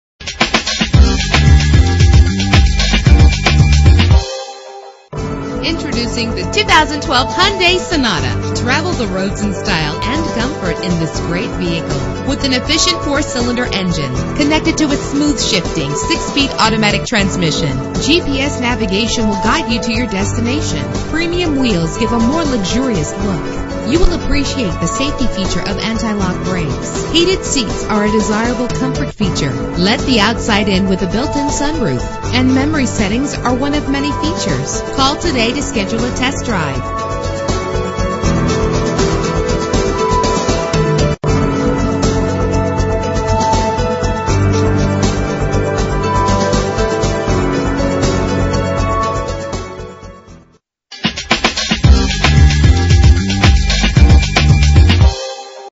2012 Hyundai Sonata - Bloomington IL
Year: 2012
Make: Hyundai
Model: Sonata
Trim: Limited
Engine: 2.4 liter inline 4 cylinder 16 valve
Transmission: 6-Speed Automatic
Color: Harbor Gray Metallic
Mileage: 0
Address:
1608 S. Morrissey Dr.